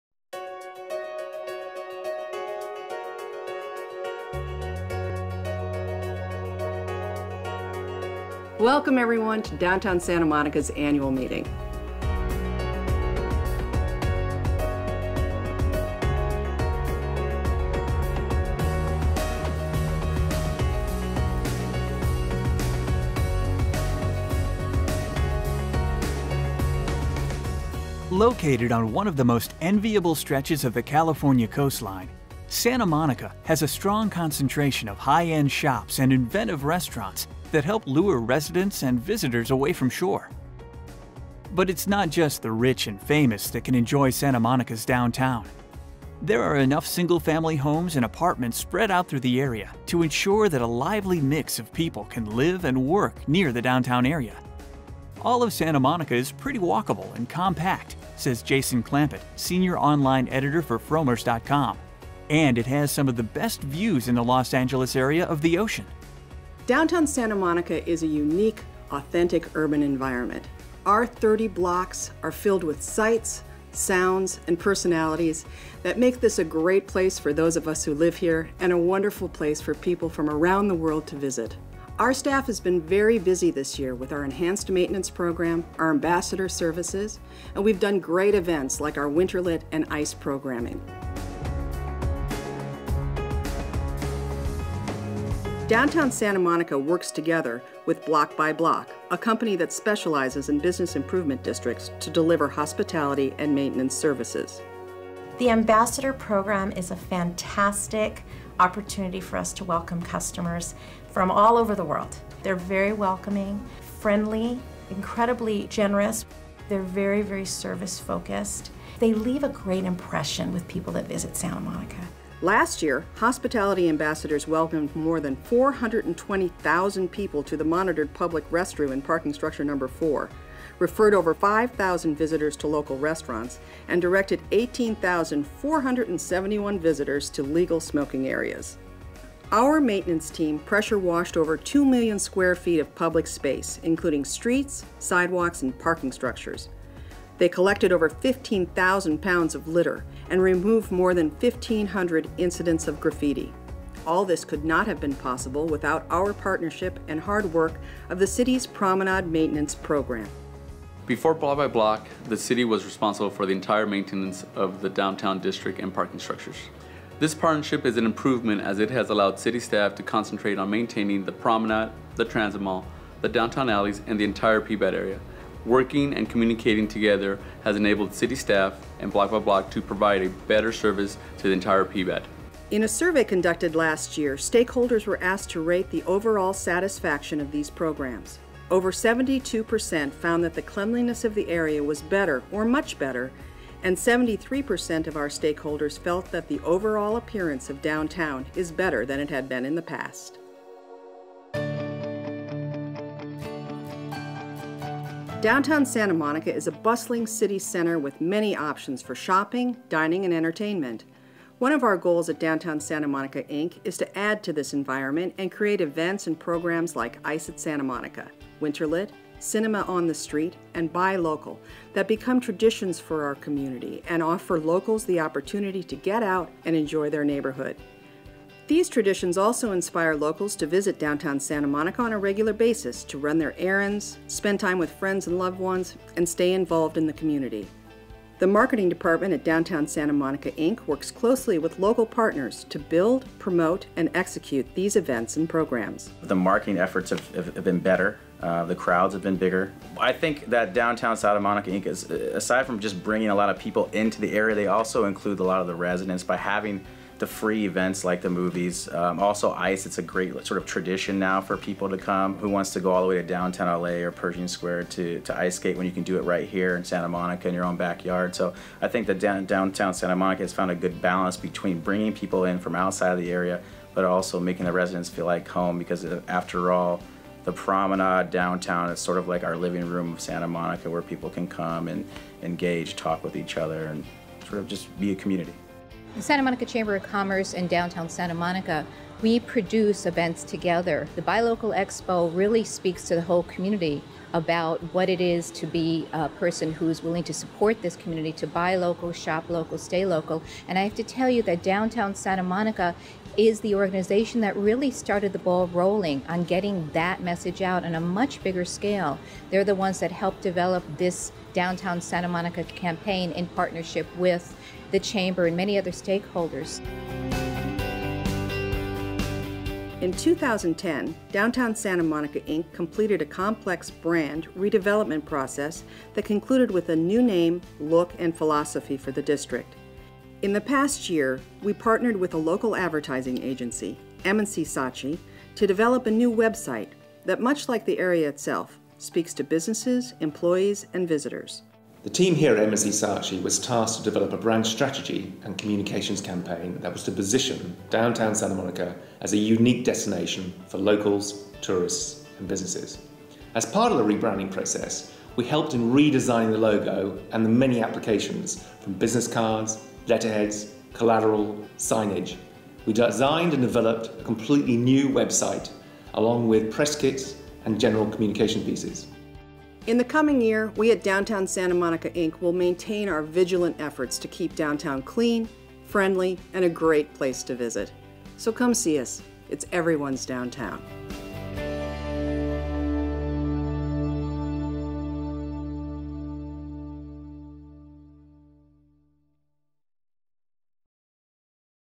2012 Annual Meeting Video - Accomplishments
On August 16, 2012 Downtown Santa Monica Inc. held their Annual Meeting presenting their accomplishments along with plans for the future of Downtown Santa Monica. This video was shown to show the operations and marketing efforts from Downtown Santa Monica Inc for the year 2011/2012.